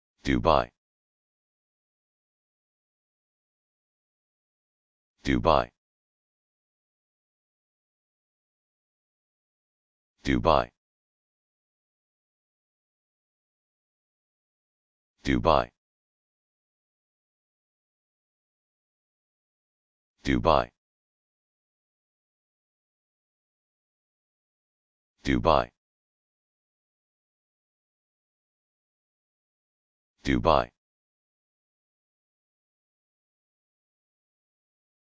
How to Pronounce Dubai
Dubai - your guide to pronouncing this English word, brought to you for free by uPronounce in an American accent.